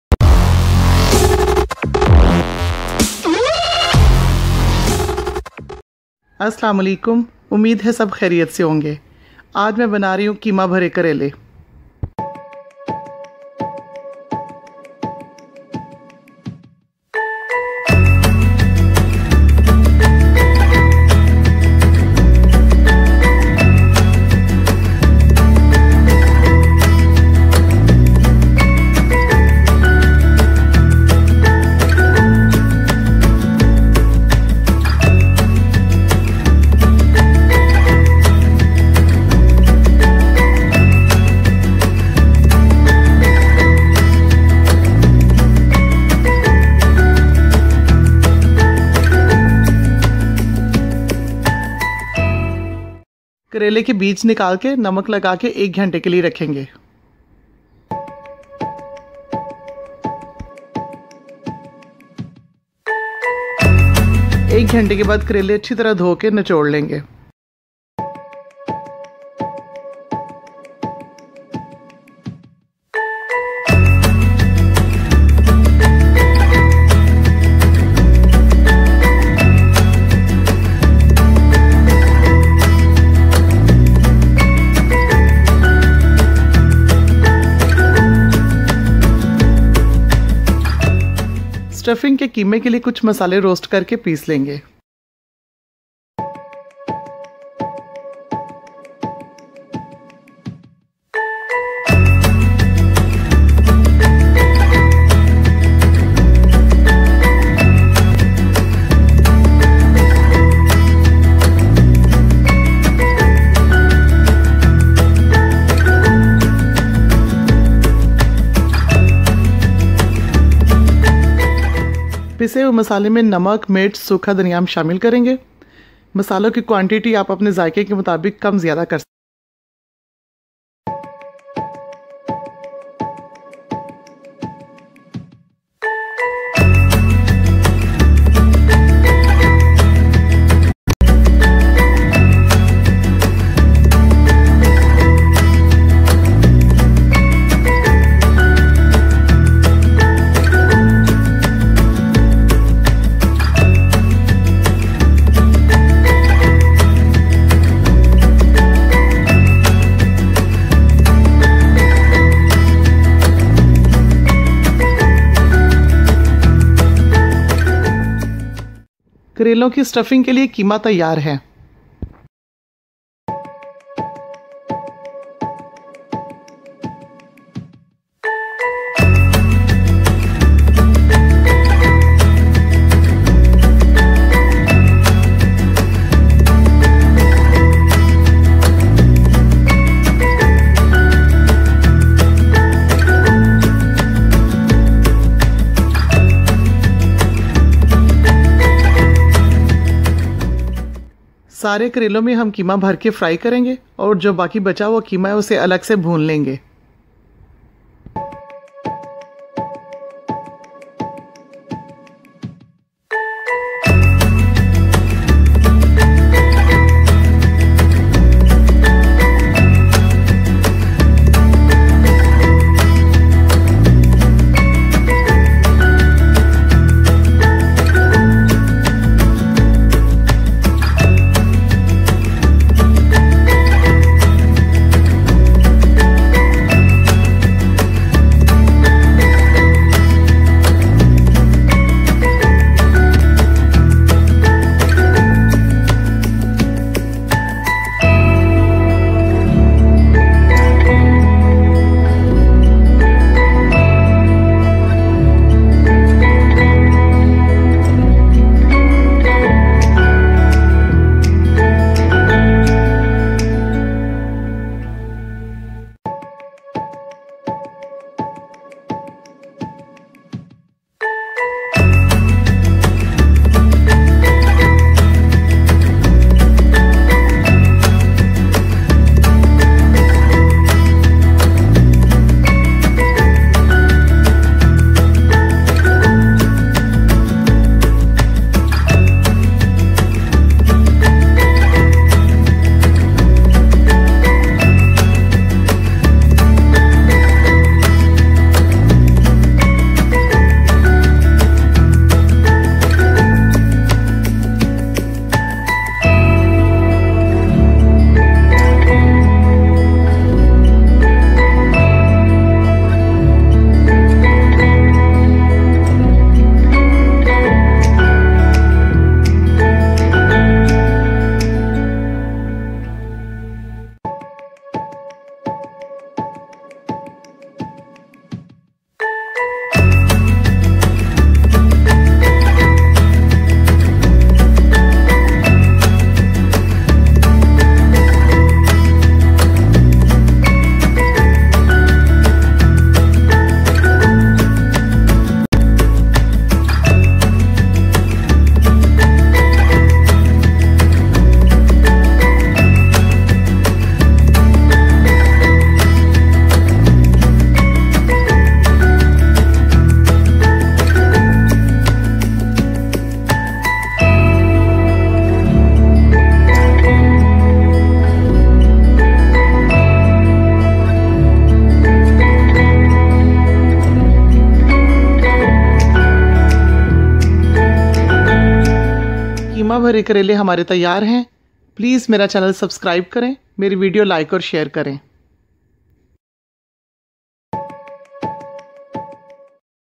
Very Tasty Qeema Bhare Karele on 2nd Day of Eid | Star Kitchen Vlogs
I made Karele stuffed with qeema on 2nd day of eid and it was very delicious. i am sure you will also like it very much and don't forget to like, share and comment.